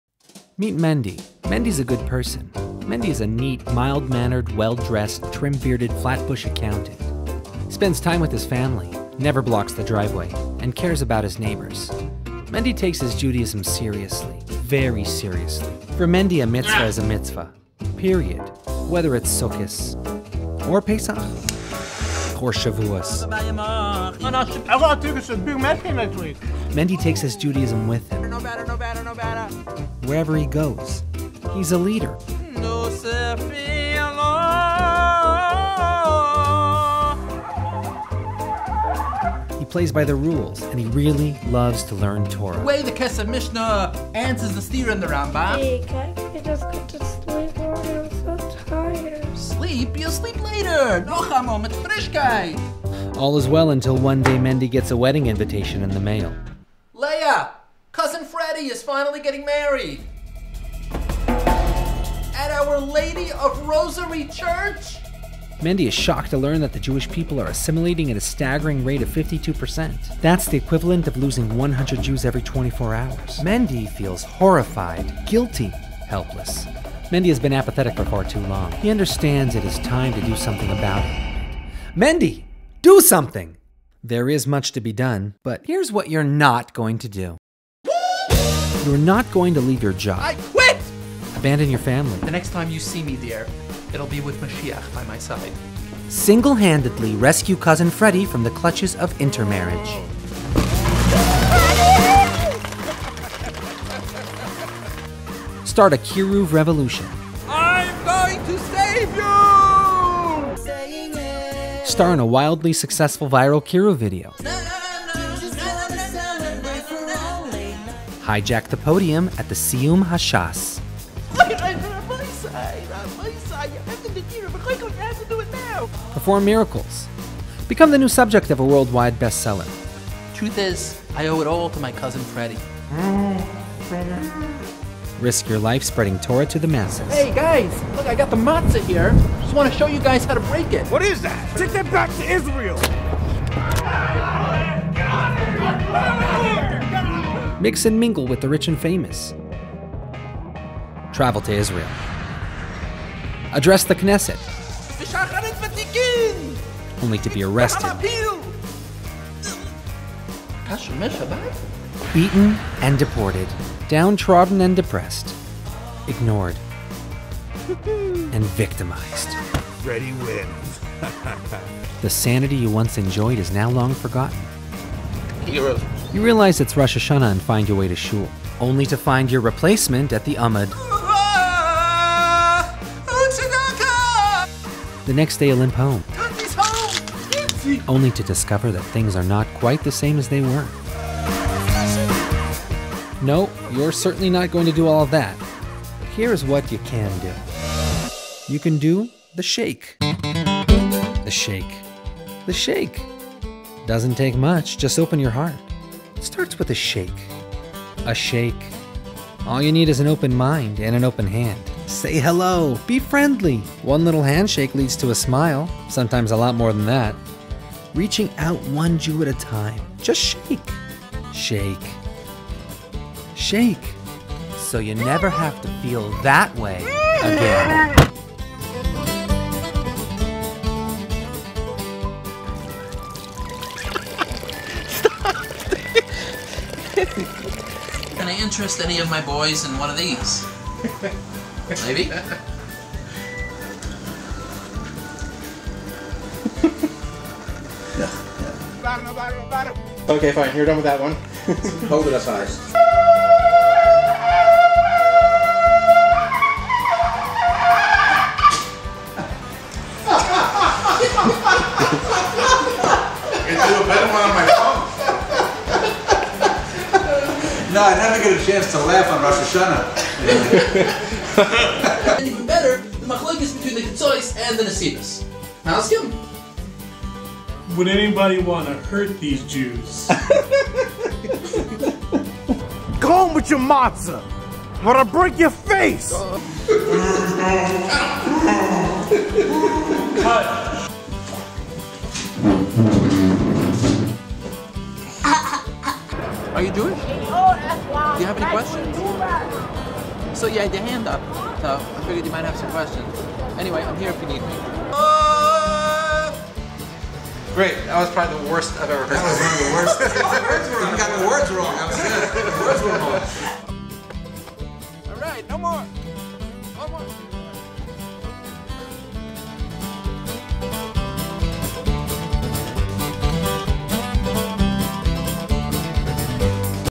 The Shake
You don't have to go to the extreme to do kiruv.
All you have to do is something as simple as, The Shake.
Small things make big things happen.

Project Inspire is a program or Aish International and their goal is to educate the Jewish Orthodox community on how to do kiruv. for more info go